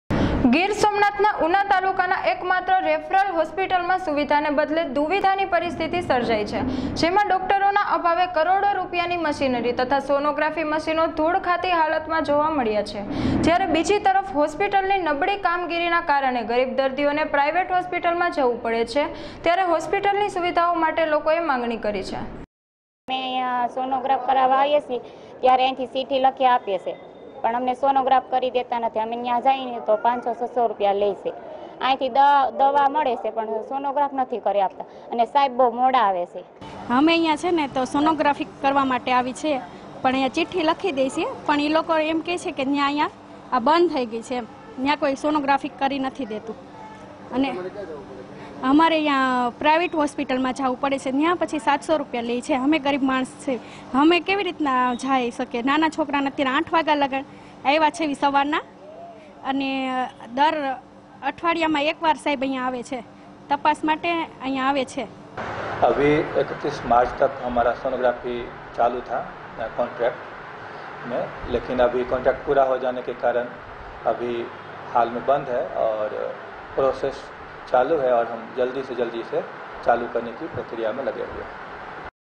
GIR SOMNATH: REFRAL HOSPITAL OF UNA DOESNT'T HAVE A PRPER FACILITY
GIR SOMNATH: REFRAL HOSPITAL OF UNA DOESNT'T HAVE A PRPER FACILITYABTAK CHANNEL is 24x7 Entertainment channel provides to its users intensive live coverage with feature shows as well.